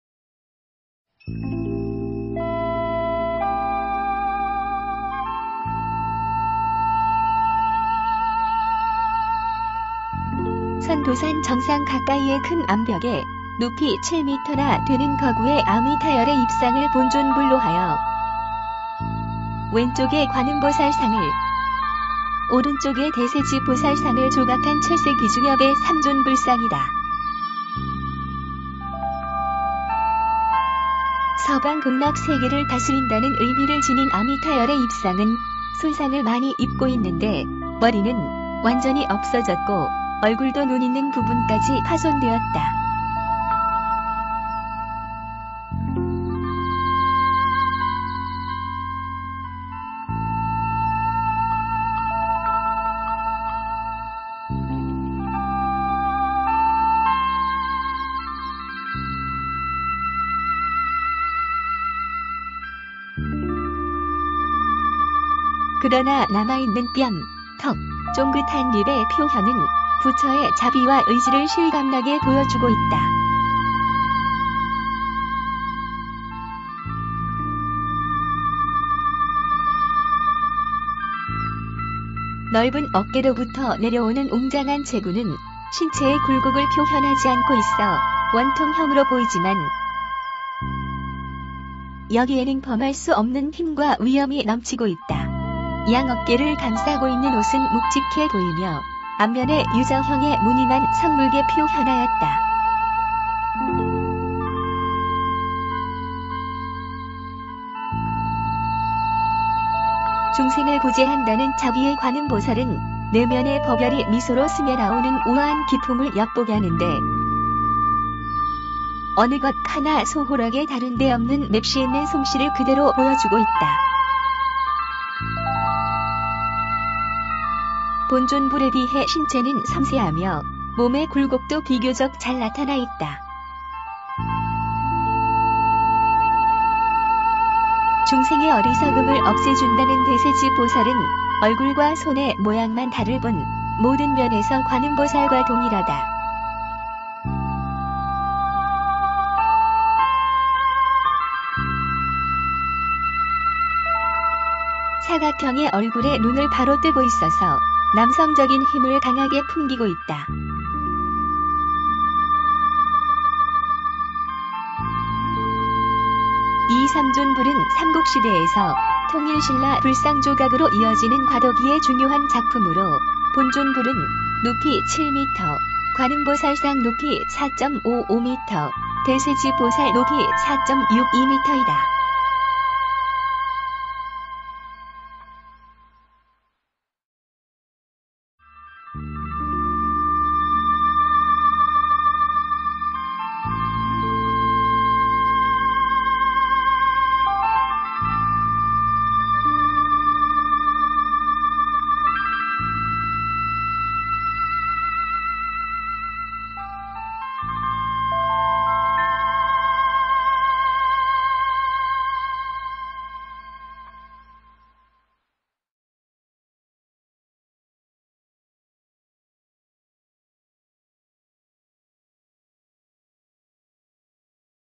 경주 서악동 마애여래삼존입상(慶州 西岳洞 磨崖如來三尊立像)
경주 서악동 마애여래삼존입상(慶州 西岳洞 磨崖如來三尊立像. 보물. 경상북도 경주시 서악동 산 92-1)
선도산 정상 가까이의 큰 암벽에 높이 7m나 되는 거구의 아미타여래입상을 본존불로 하여, 왼쪽에 관음보살상을, 오른쪽에 대세지보살상을 조각한 7세기 중엽의 삼존불상(三尊佛像)이다.
 서방 극락세계를 다스린다는 의미를 지닌 아미타여래입상은 손상을 많이 입고 있는데, 머리는 완전히 없어졌고 얼굴도 눈 있는 부분까지 파손되었다. 그러나 남아있는 뺨, 턱, 쫑긋한 입의 표현은 부처의 자비와 의지를 실감나게 보여주고 있다. 넓은 어깨로부터 내려오는 웅장한 체구는 신체의 굴곡을 표현하지 않고 있어 원통형으로 보이지만, 여기에는 범할 수 없는 힘과 위엄이 넘치고 있다. 양 어깨를 감싸고 있는 옷은 묵직해 보이며, 앞면에 U자형의 무늬만 성글게 표현하였다.
 중생을 구제한다는 자비의 관음보살은 내면의 법열(法悅)이 미소로 스며 나오는 우아한 기풍을 엿보게 하는데, 어느 것 하나 소홀하게 다룬 데 없는 맵시 있는 솜씨를 그대로 보여주고 있다. 본존불에 비해 신체는 섬세하며 몸의 굴곡도 비교적 잘 나타나 있다. 중생의 어리석음을 없애준다는 대세지보살은 얼굴과 손의 모양만 다를 뿐 모든 면에서 관음보살과 동일하다. 사각형의 얼굴에 눈을 바로 뜨고 있어서 남성적인 힘을 강하게 풍기고 있다.
 이 삼존불은 삼국시대에서 통일신라 불상조각으로 이어지는 과도기의 중요한 작품으로, 본존불은 높이 7m, 관음보살상 높이 4.55m, 대세지보살 높이 4.62m이다.(출처 : 문화재청)